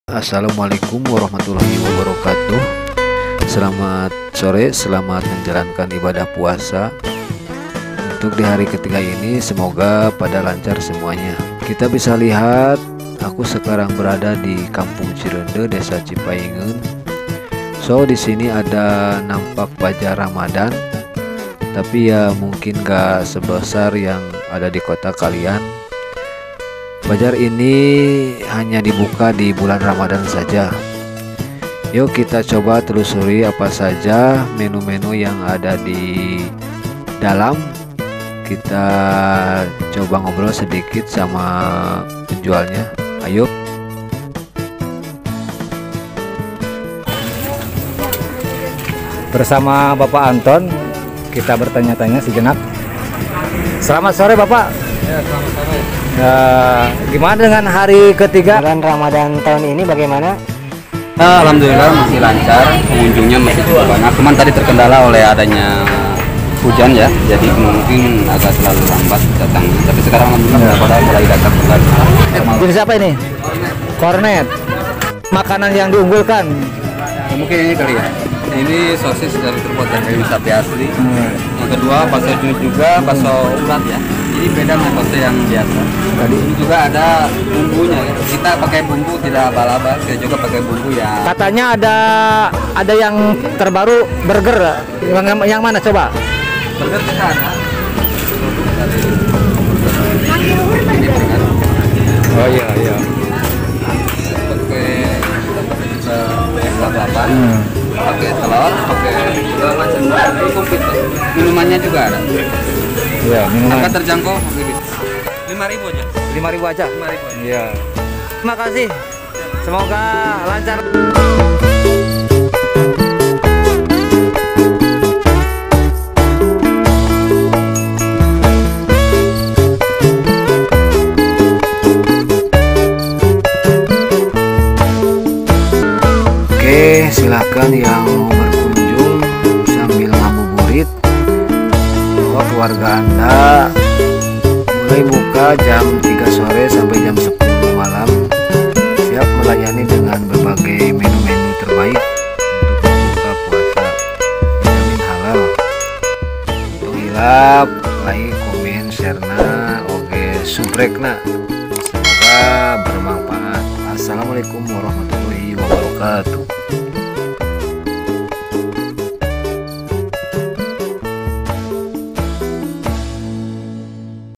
BAZAR RAMADHAN Di kampung juga ADA
lokasi kp cireunde desa cipaingeun kec sodonghilir Tasikmalaya
dalam setiap tahun tepatnya di bulan Ramadhan
tempat ini selalu di jadikan dimana para pedagang menjajakan dagangannya
berbagai olahan makanan untuk di jadikan menu berbuka puasa

semoga menjadi inspirasi buat daerah2 lain bisa di jadikan Contoh , mengangkat ekonomi kreatif masyarakat sekitar terimakasih